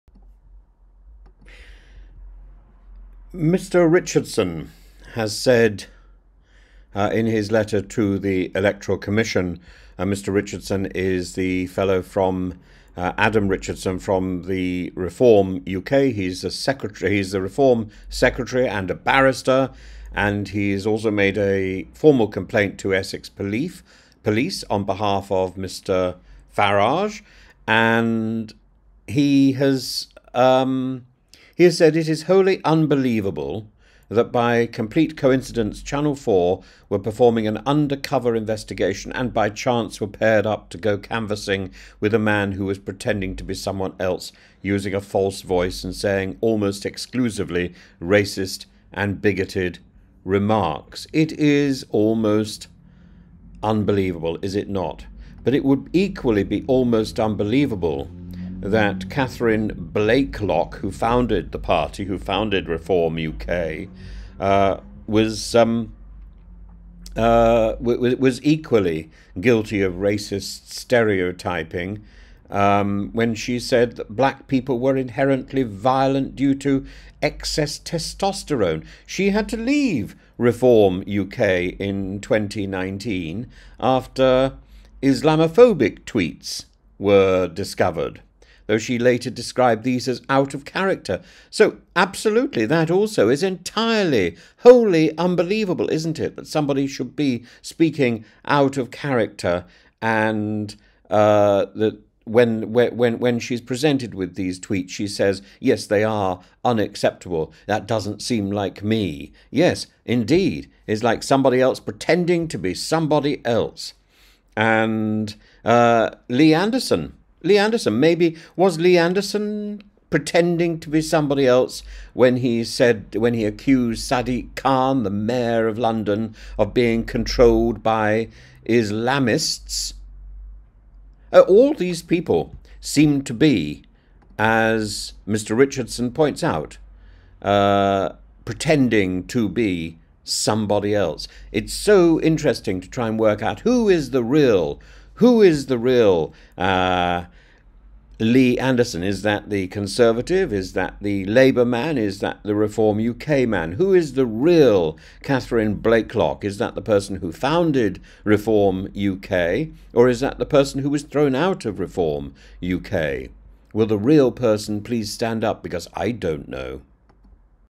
who is the "Real person" in Reform uk?
Adam Richardson the secretary of Reform UK has written to the Essex Police and also to the Electoral commission because he seems unable to identify what Reality might be. Poor man. He should have a chat with Lee Anderson or the founder of Reform UK, Catherine Blaiklock.